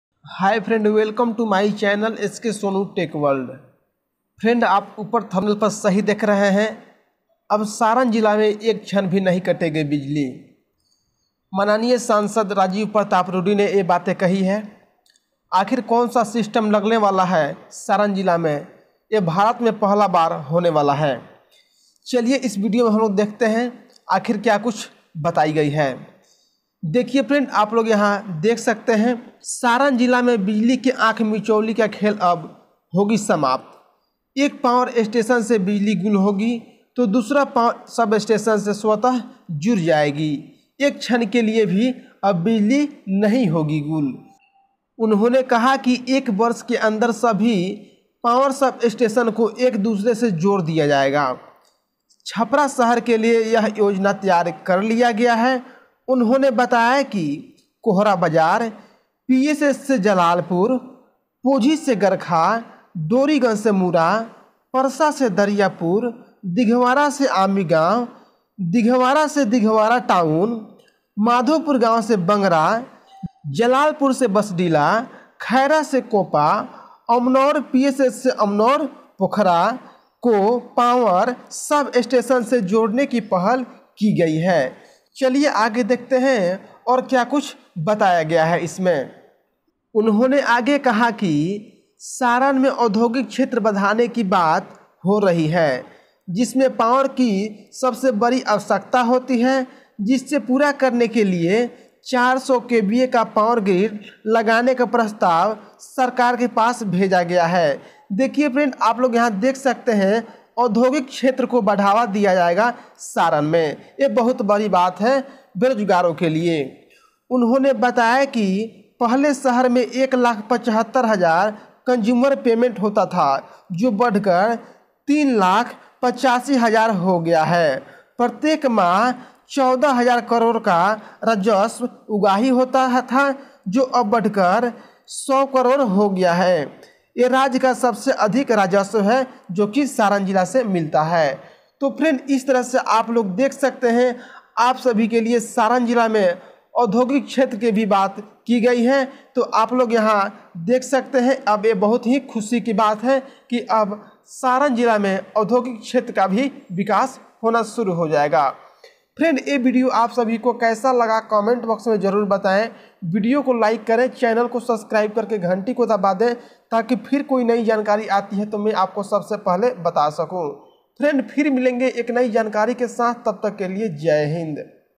सारन जिला में अब एक क्षण भी बिजली नहीं होगी - सांसद राजीव प्रताप रूडी | saran district electric
about: It has been told in this video that if there is a line anywhere in the district, then that line will automatically go.